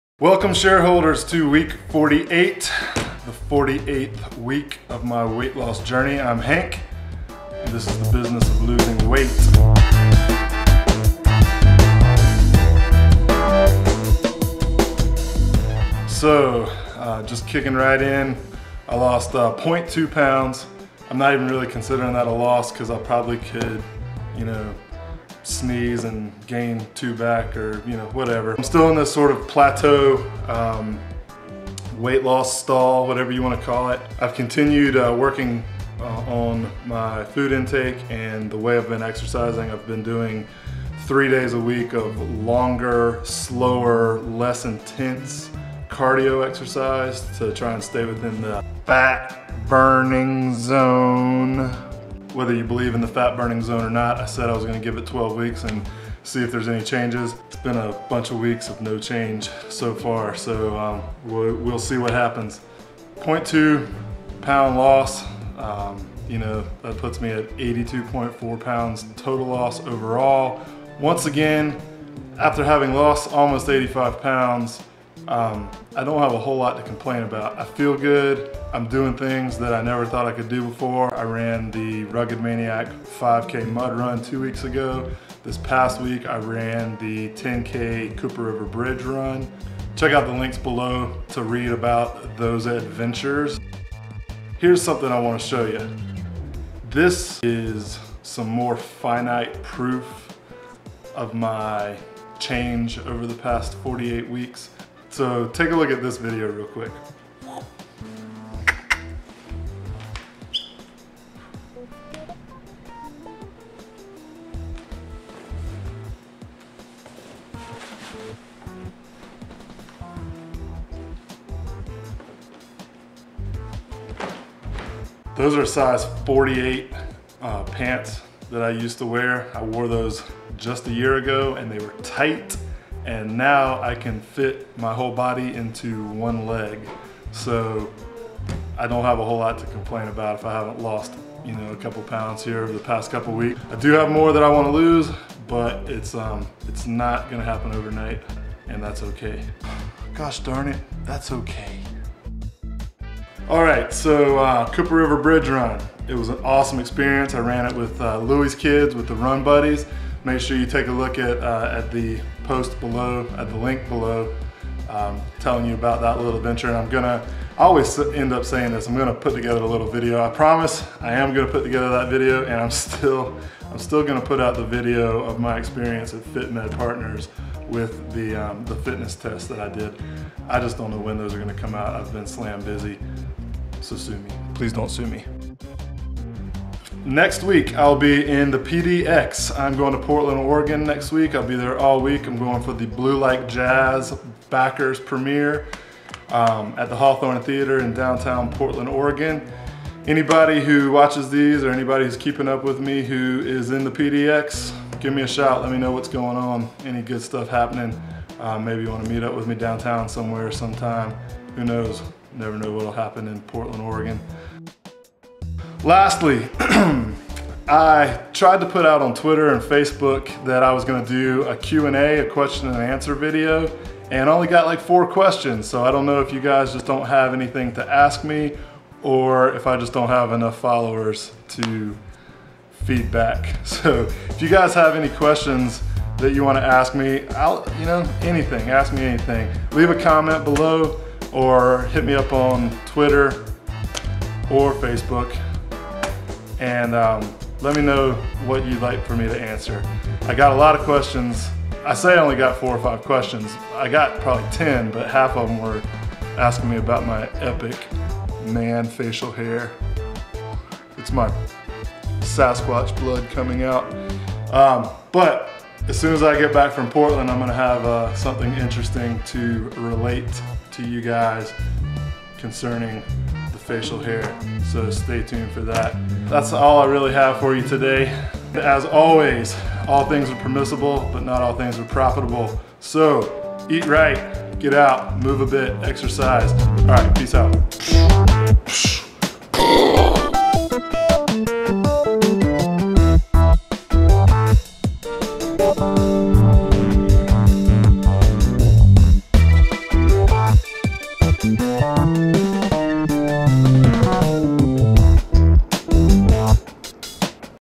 Week 48 Weigh In - Business of Losing Weight
This is the first weigh in for the rest of my life. Welcome to the 47th weigh in in my weight loss journey. Visit the progress page on my blog if you want to see an ongoing chart of my journey.

Here are some mentioned links:
My blog about running the Rugged Maniac

My blog about running the Cooper River Bridge Run